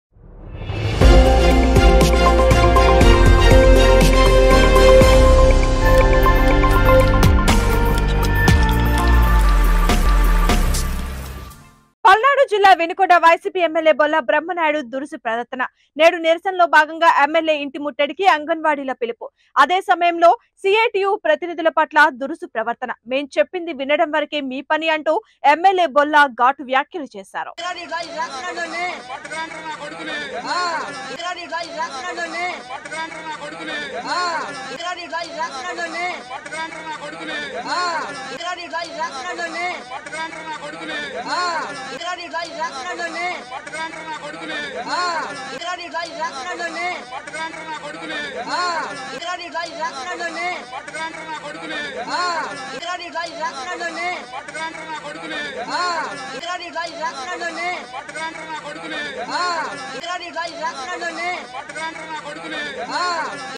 బొల్లా బ్రమ్మనాయుడుదురుసు ప్రవర్తననేడు నిరసనలో భాగంగా ఎమ్మెల్యే ఇంటి ముట్టడికి అంగన్వాడీలపిలుపుmitv
వినుకొండ వైసీపీ ఎమ్మెల్యే బొల్లా బ్రమ్మనాయుడు దురుసు ప్రవర్తన
నేడు నిరసనలో భాగంగా ఎమ్మెల్యే ఇంటి ముట్టడికి అంగన్వాడీల పిలుపు